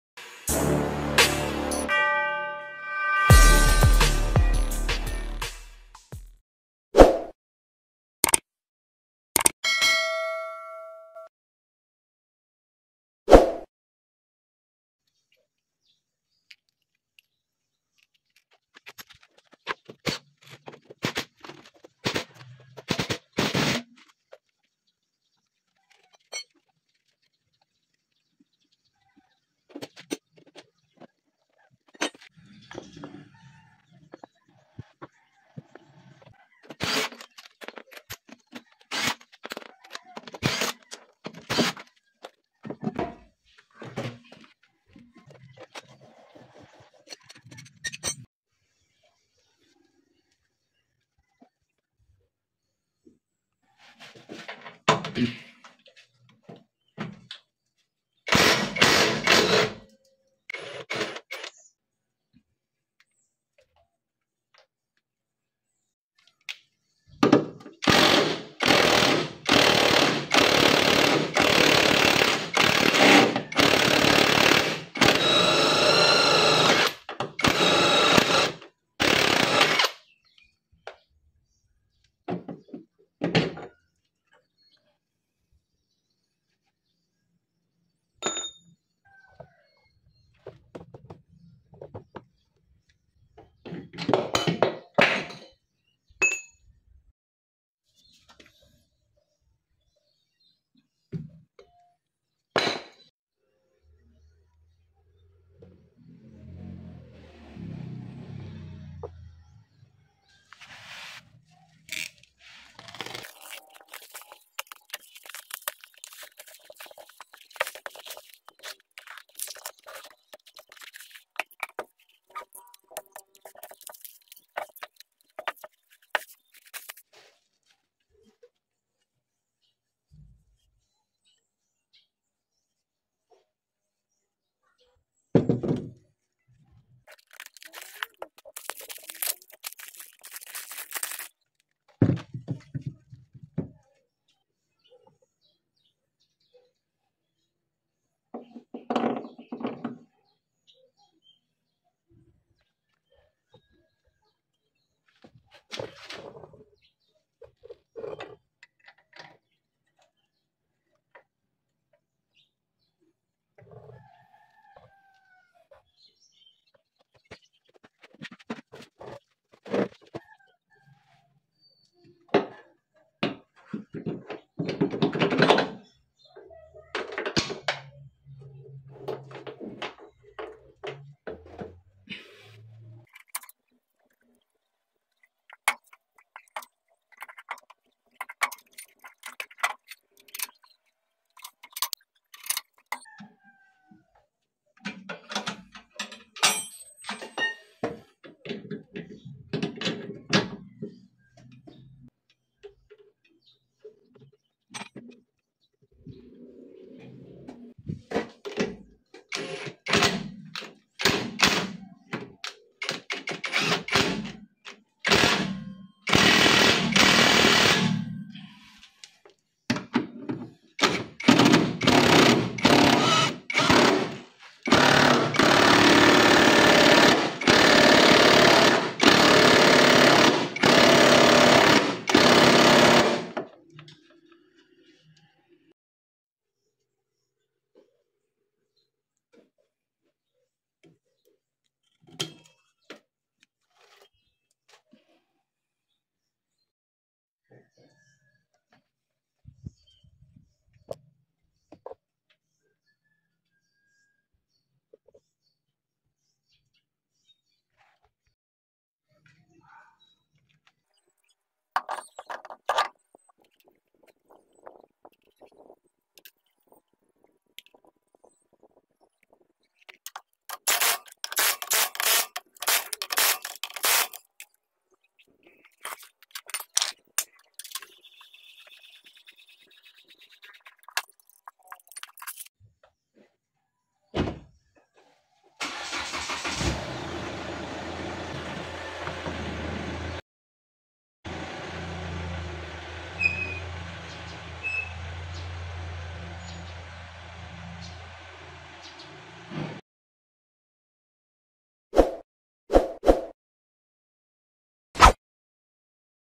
Hyundai Accent Rear SHOCK ABSORBER Replacement
Installation of new rear absorber to accent.